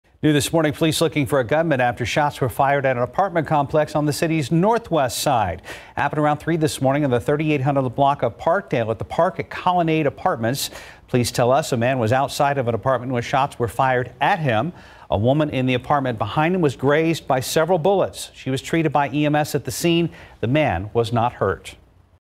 Woman grazed by gunfire at NW Side apartment, police say
San Antonio police are investigating after a woman was grazed by gunfire at a Northwest Side apartment complex early Friday morning.